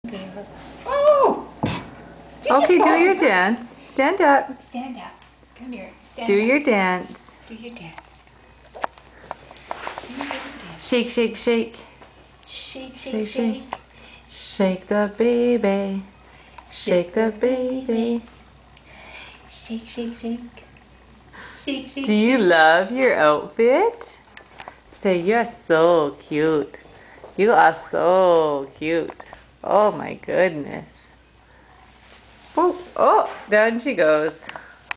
indian babe
awnya tries on, and models her indian outfit from her aunty jen!